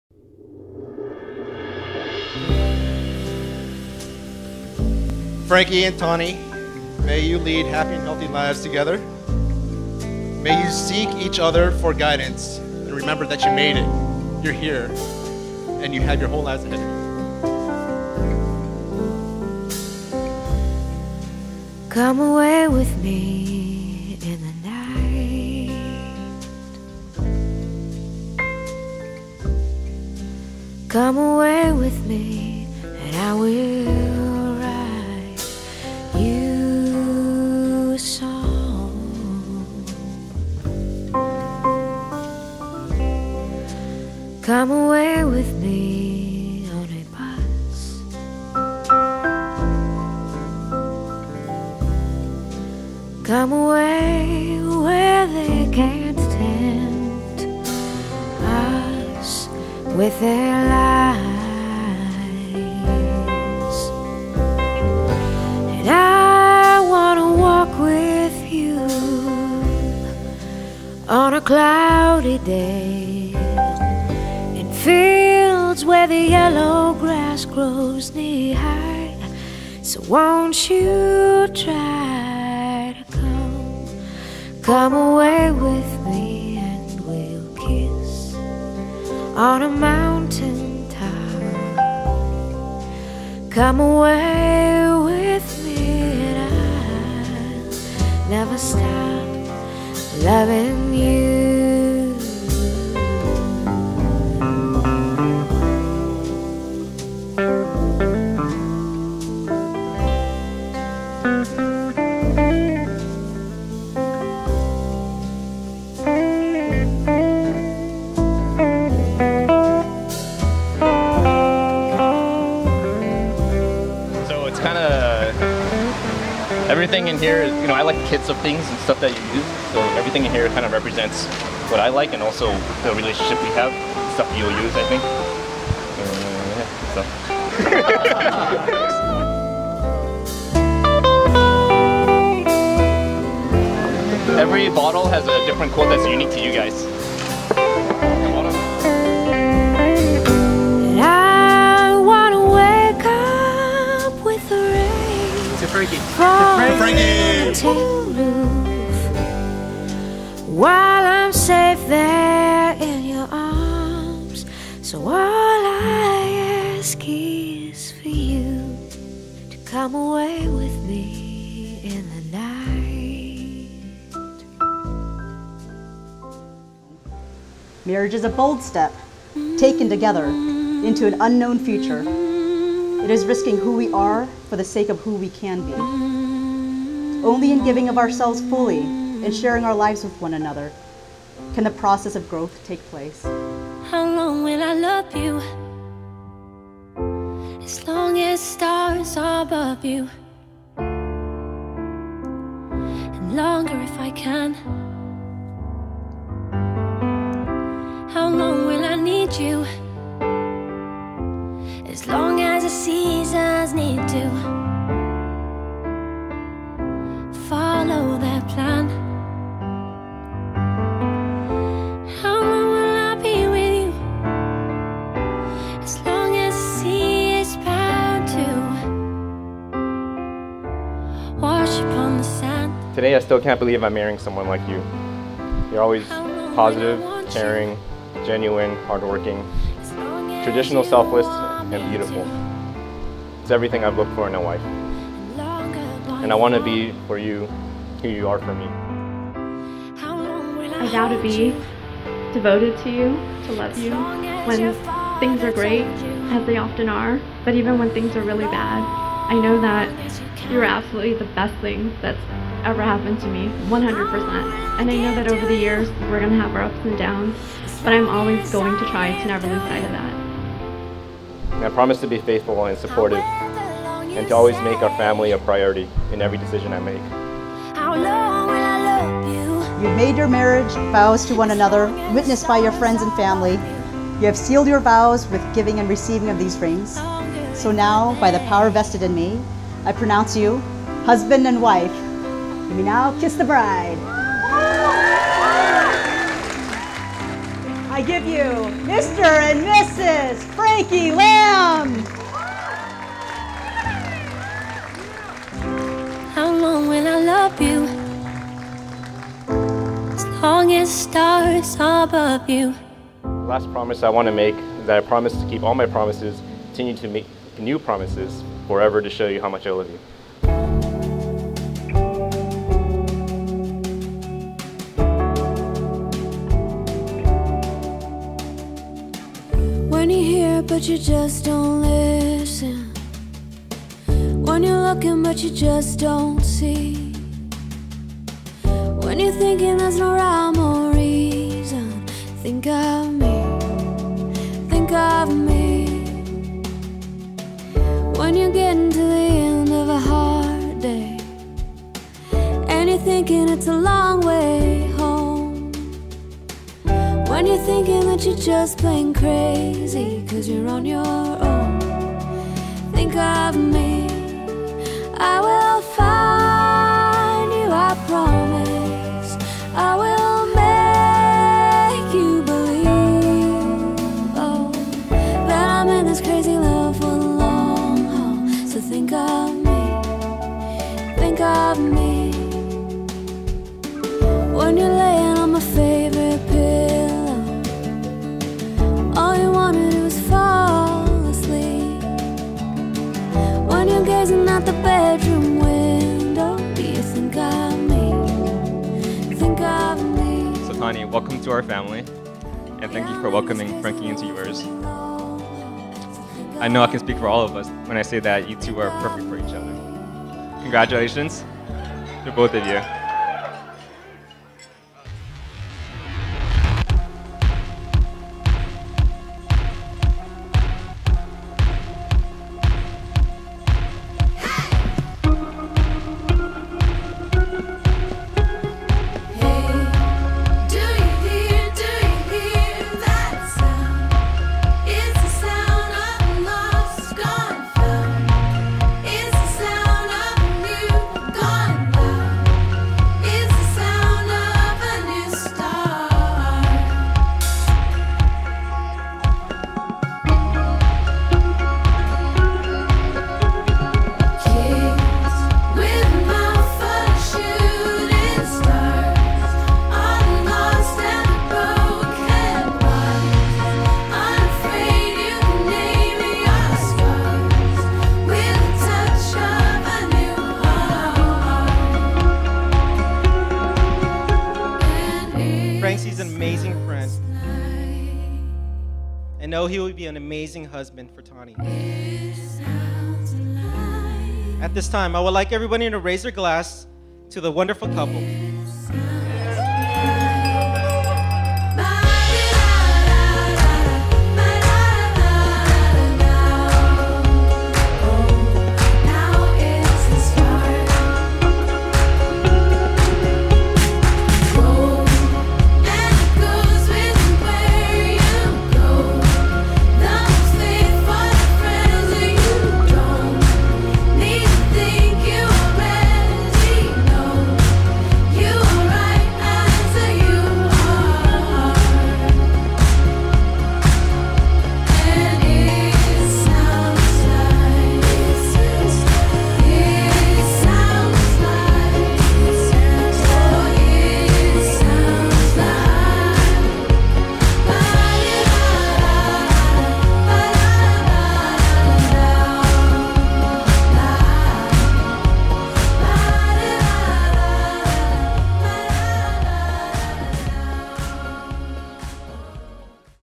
Tawnee Huynh & Frankie Lam - Wedding Day Highlight
On July 02, 2016, Tawnee and Frankie became husband and wife.  They chose to have their wedding at the Calamigos Resort & Spa located in Malibu, California.They held a beautiful, outdoor Christian Ceremony under a gazebo where they exchanged vows in front of family and friends. The reception followed in the evening in the main ballroom.

Coordination by: Sheryl